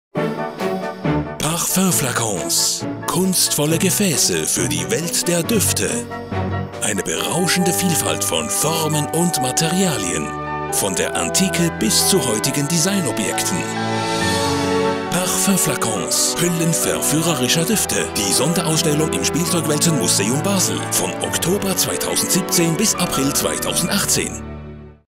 Sonderausstellung «Parfumflacons – Hüllen verführerischer Düfte»
Die Sammlung Storp – von der Antike bis heute
21. Oktober 2017 – 8. April 2018

Ein Tag kann so schön sein: Ein paar Tropfen aus einem schönen Flakon oder ein leichter Druck auf den Zerstäuber – und schon umgibt einen eine wahre Wolke Wohlgefühl. Düfte sind ein kleiner Luxus und sie bedürfen eines entsprechenden Behältnisses.

Schon bei den alten Ägyptern war der Wunsch verbreitet, einen Duftstoff permanent bei sich tragen zu können. Da ein Duft nur wirken konnte, wenn man ihn stets in Reichweite hatte, benötigte man geeignete Aufbewahrungsbehälter.

Flakons waren dazu bestimmt, jedem Parfum eine ganz eigene, persönliche Note zu verleihen. Heute werden diese Designobjekte als Kunstwerke anerkannt. Sie waren immer den Einflüssen des Zeitgeistes unterworfen. Flakons gehören zu den wichtigsten Marketingwerkzeugen der Beautyindustrie.

In dieser Sonderausstellung sind über 500 Flakons aus den letzten 5000 Jahren zu sehen. Die Vielfalt der Formen ist überwältigend und den Materialien sind praktisch keine Grenzen gesetzt: Gold, Silber, Glas, Porzellan, Email, Schildpatt, Perlen oder Edelsteine. Die ausgestellten Flakons der Storp Collection München sind lediglich ein Auszug aus der weltgrössten Privatsammlung.

In der Ausstellung begegnet man auch Düften aus längst vergangener Zeit, welche von unseren heutigen Nasen nicht mehr unbedingt als wohlriechend wahrgenommen werden. Dazu kommen zwölf Düfte aus verschiedenen Duftfamilien. Workshops und Wettbewerb geben kleinen und grossen Besuchern Gelegenheit, die Welt der Flakons und Parfums selber zu erleben.